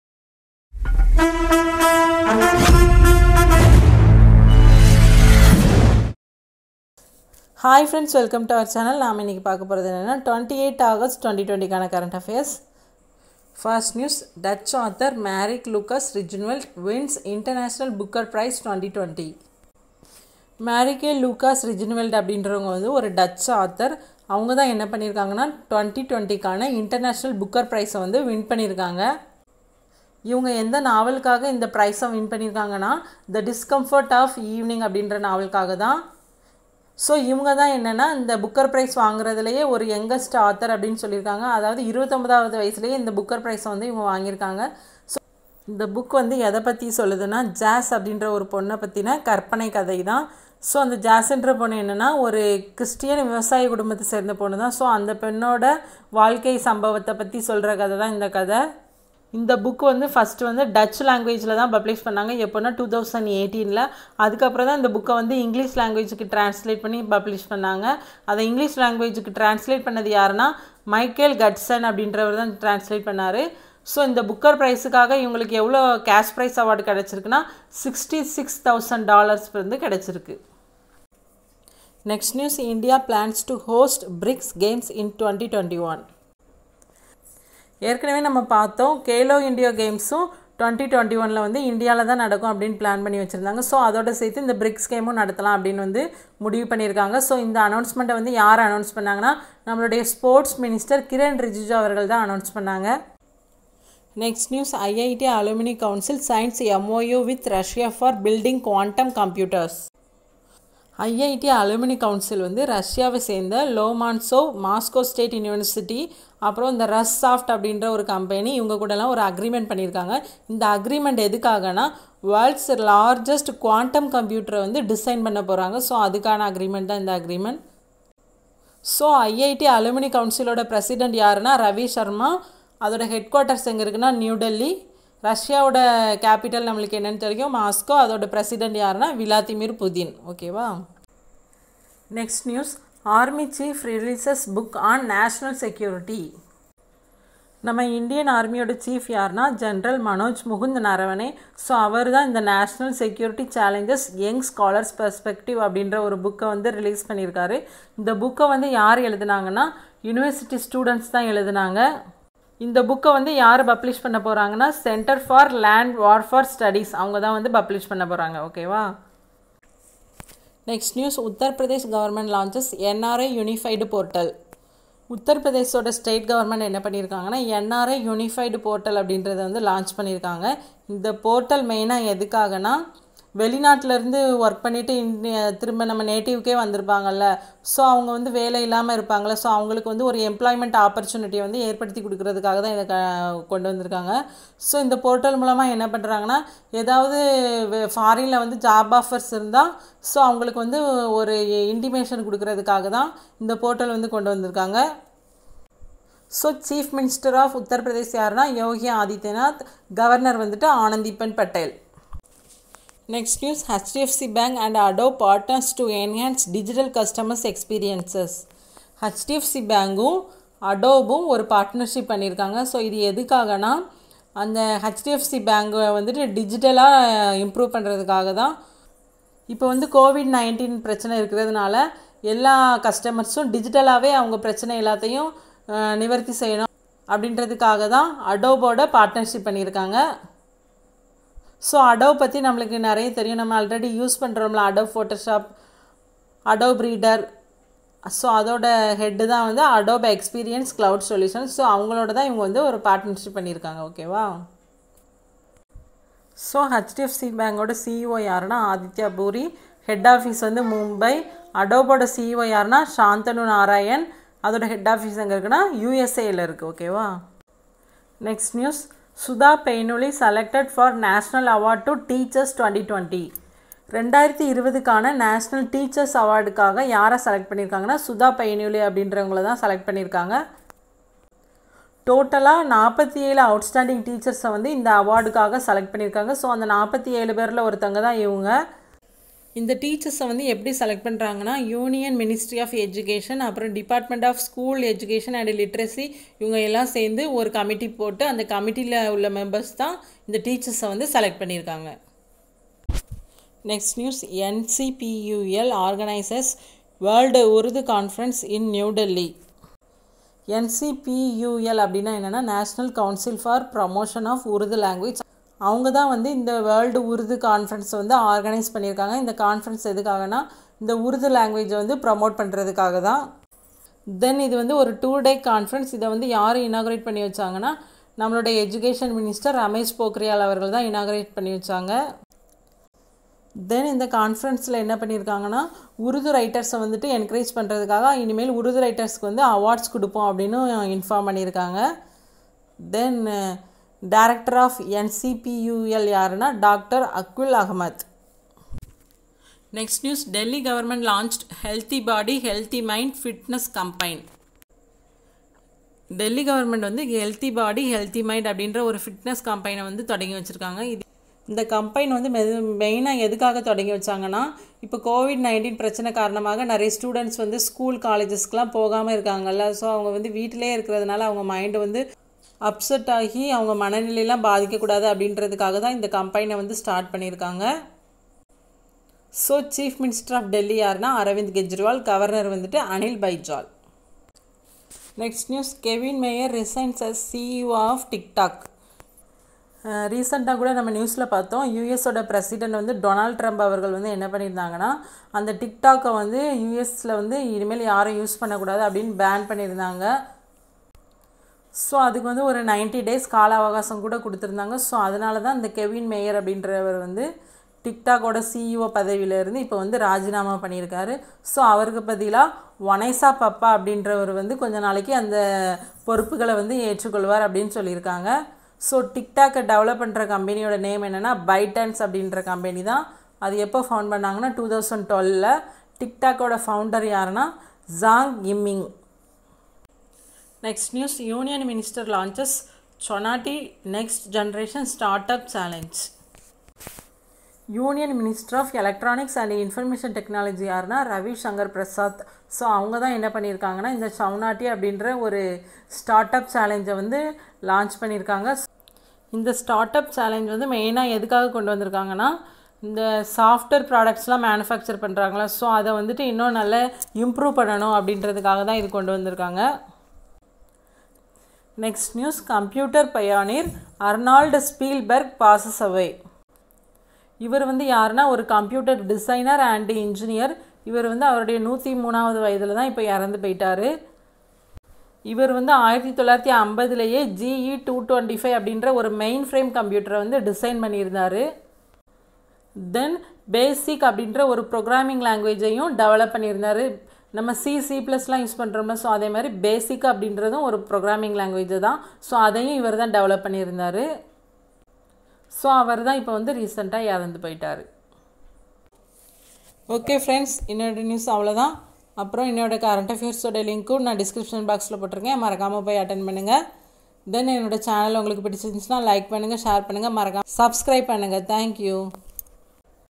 Daily current affairs in tamil 28 Aug 2020 | 28.08.2020| RRB, TNPSC, BANK, SSC, Railway
Today's Quiz